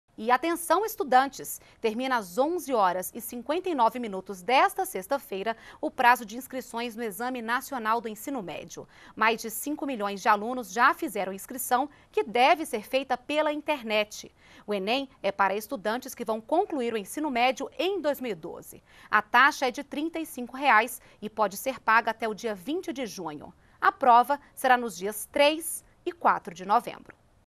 Terminam as inscrições para o Enem
NBR NOTÍCIAS - 15.06.12: Terminou nesta sexta-feira (15), às 23 horas e 59 minutos, o prazo de inscrições no Exame Nacional do Ensino Médio. Até o início da noite desta sexta, mais de 5 milhões de estudantes fizeram a inscrição pela internet. O Enem é para estudantes que vão concluir o ensino médio em 2012. A taxa é de R$ 35 e pode ser paga até o dia 20 de junho. O Enem será nos dias 3 e 4 de novembro. Informações no